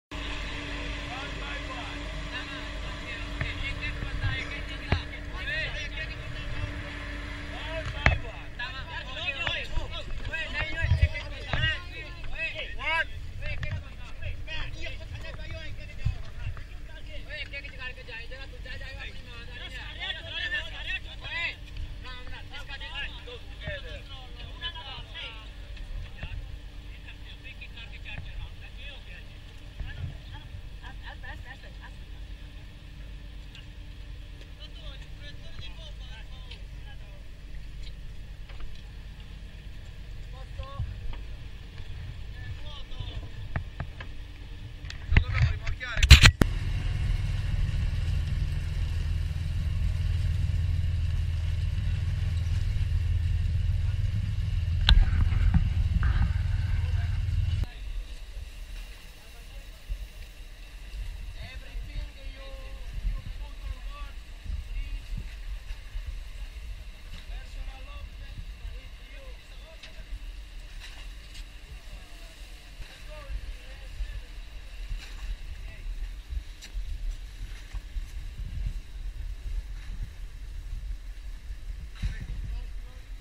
2016 03 06 soccorso cp292 kos ok
Roma 6 Marzo: video delle operazioni di salvataggio condotte la scorsa notte nel Mare Egeo dalla Motovedetta CP 292 della Guardia Costiera italiana, che ha soccorso 13 migranti, di origine pachistana ed afghana, a bordo di un gommone semi affondato alla deriva nelle acque di Kos.